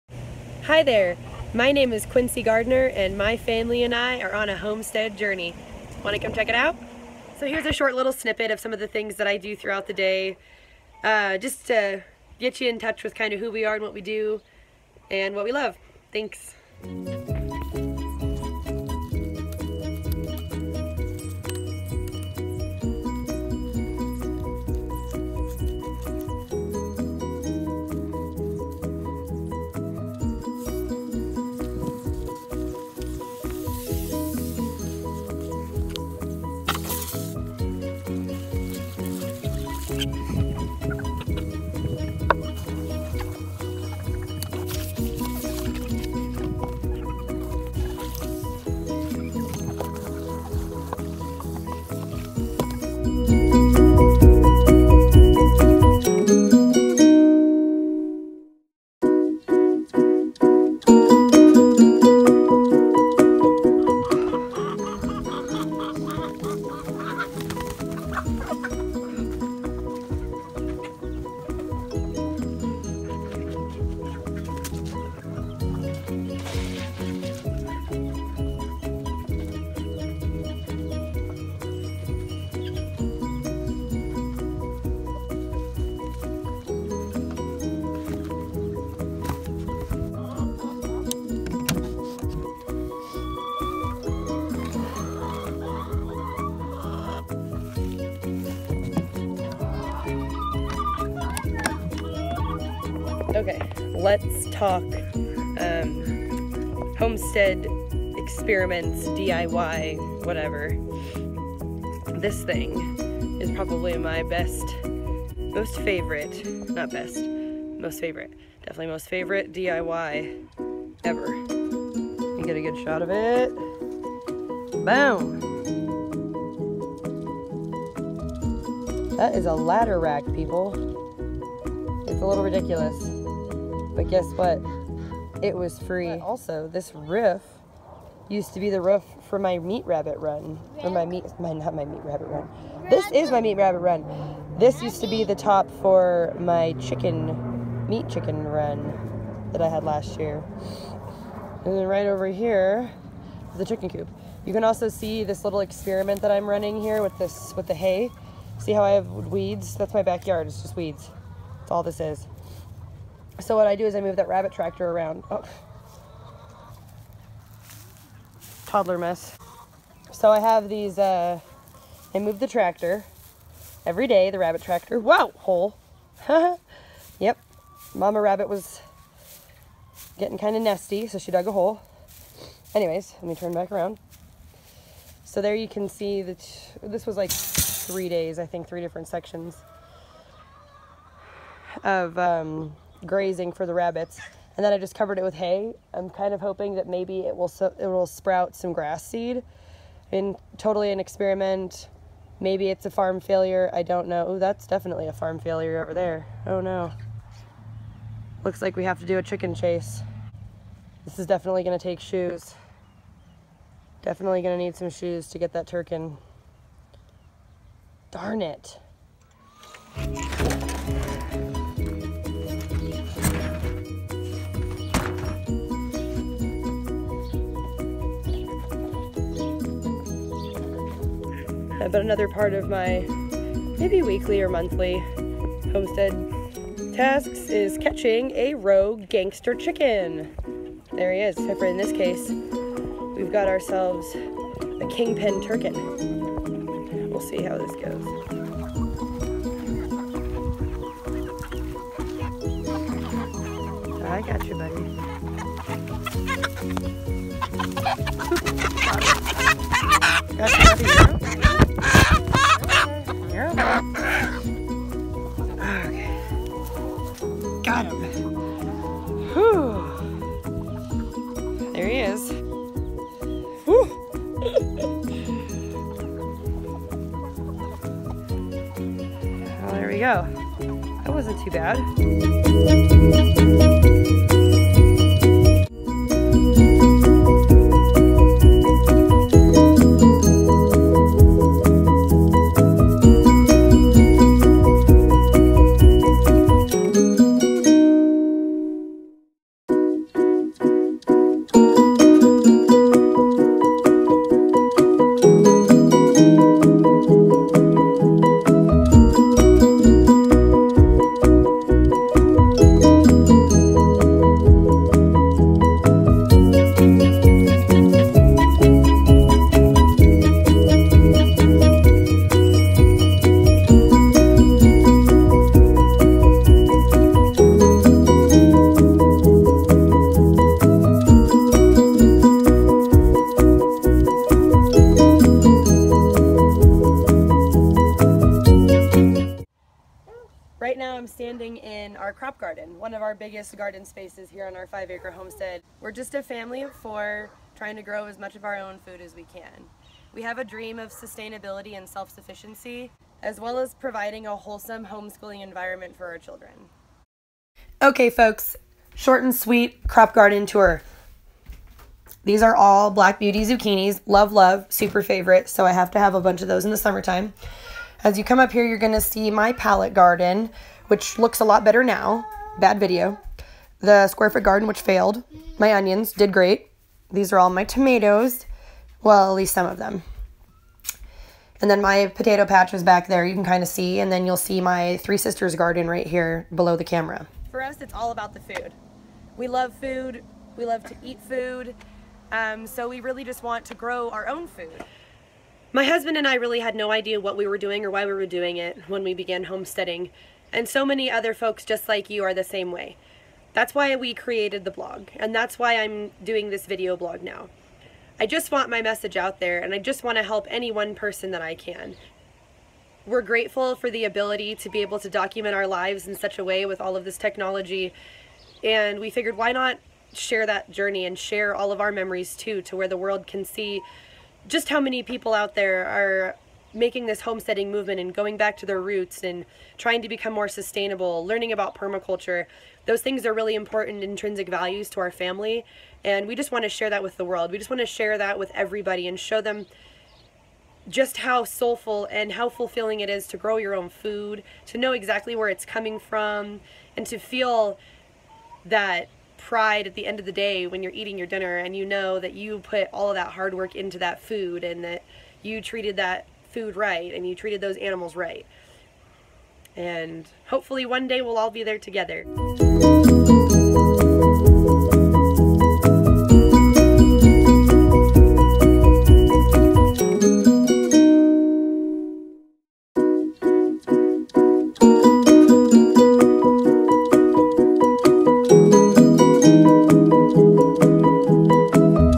Pilot Vlog. Welcome To The Homestead.
Hi there folks! Welcome to The Gardner Homestead. Our family is trying to grow our own food and get back to our roots.

I'm always open to pointers and feedback, no clue what I'm doing over here peeps!